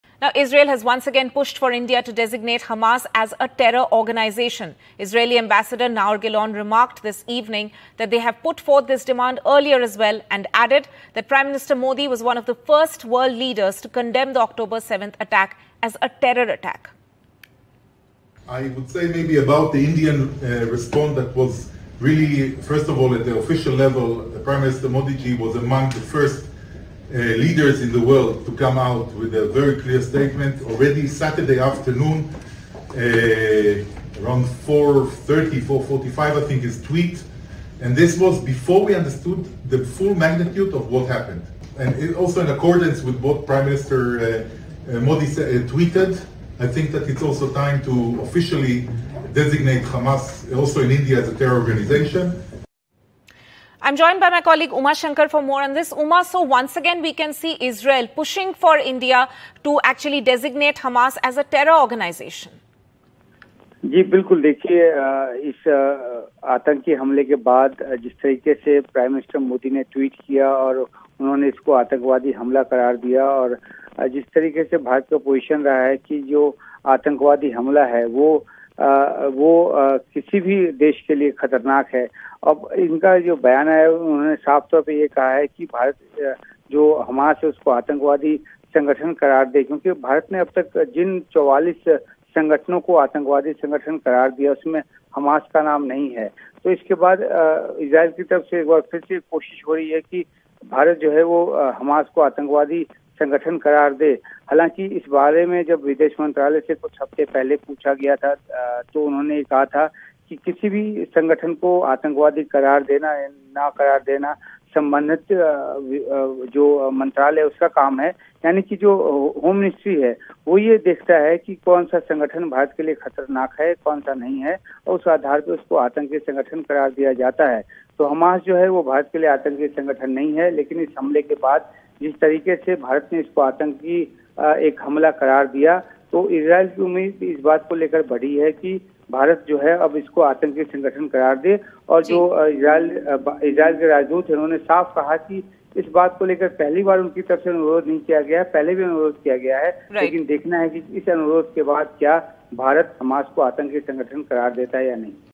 Time For India To Declare Hamas As Terror Organisation, Says Israel Envoy
The time has come for India to proscribe Hamas as a terrorist organisation like many other nations have done, Israeli ambassador Naor Gilon said on Wednesday. The Israeli envoy, at an interaction with journalists, also thanked India for its "100 per cent" support to Israel in its anti-terror operations against Hamas.